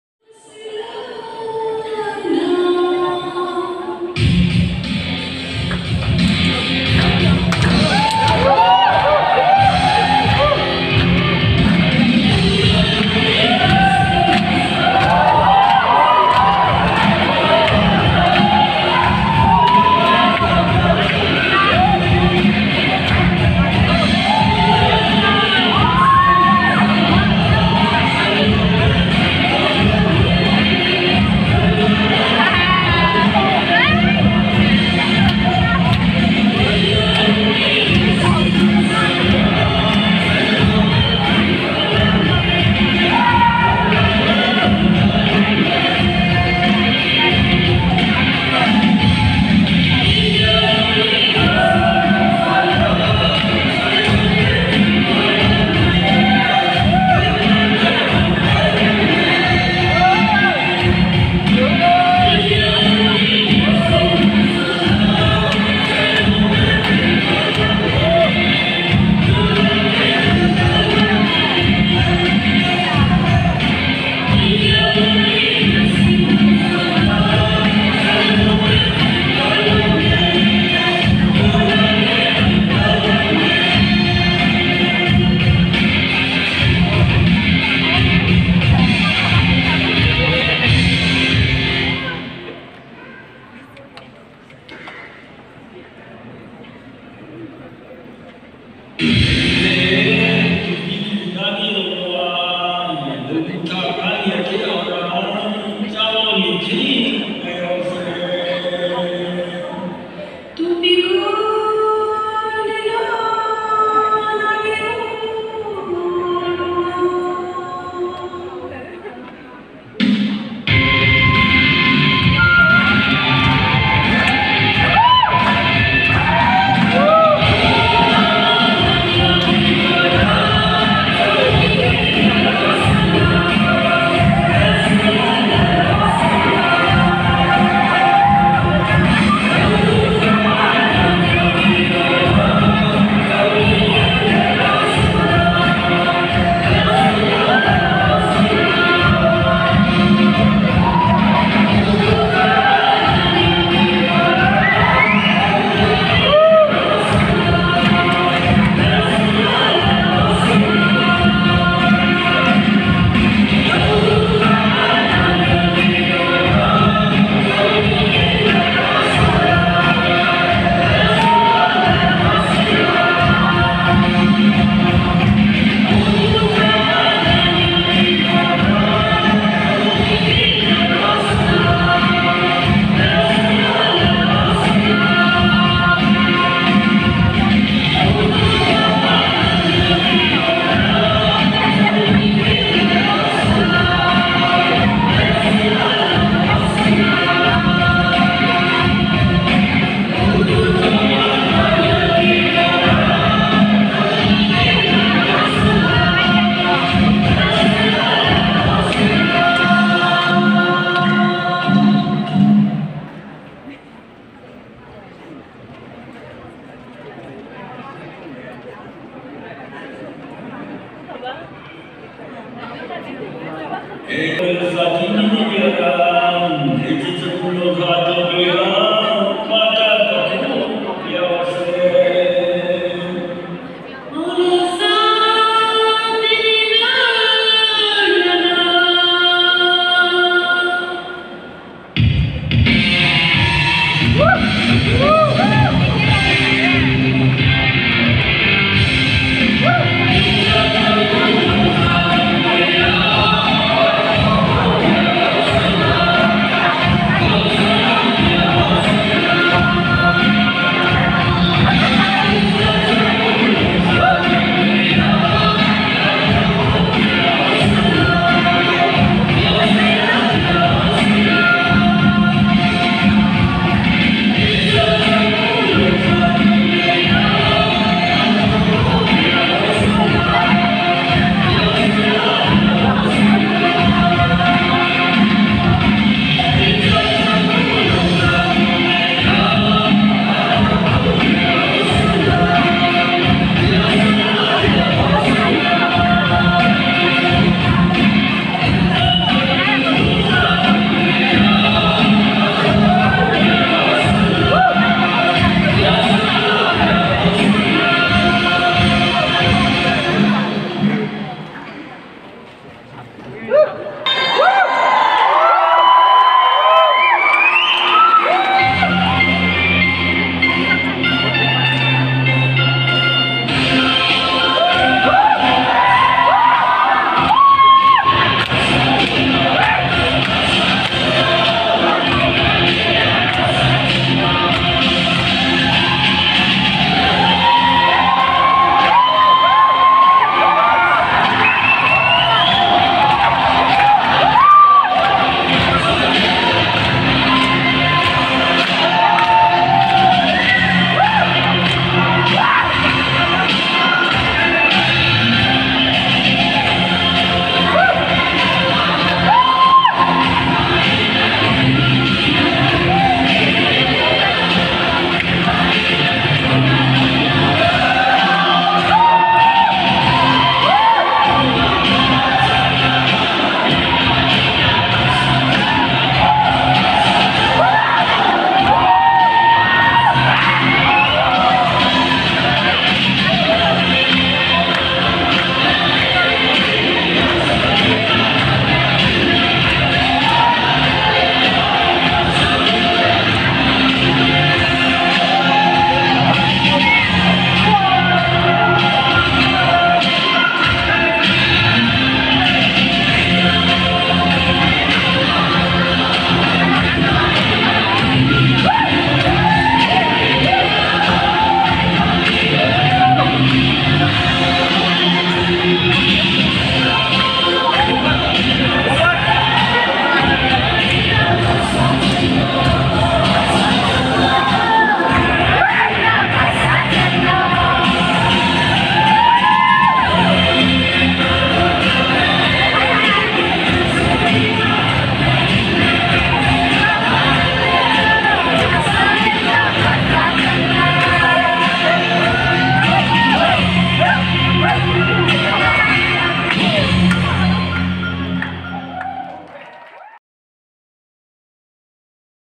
marah cultural dance "Awkhypa Lâ" performed by III Semester Govt. Saiha College, Siaha.
This is one of the most beautiful marah cultural dance. They are called as "AWKHYPA LÂ" in marah language.
If any problem with this video,please keep me in the loop immediately to YouTuber.